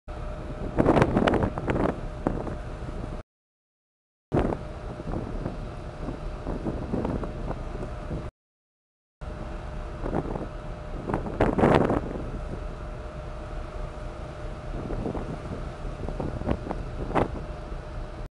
экономия судового топлива экология ecology marine fuel savings
полное отсутствие дыма при работе корабельного двигателя на тяжелом остаточном судовом топливе IFO-180 с использованием технологии TRGA

complete absence of smoke on ship engine with heavy residual marine fuel IFO-180 using technology TRGA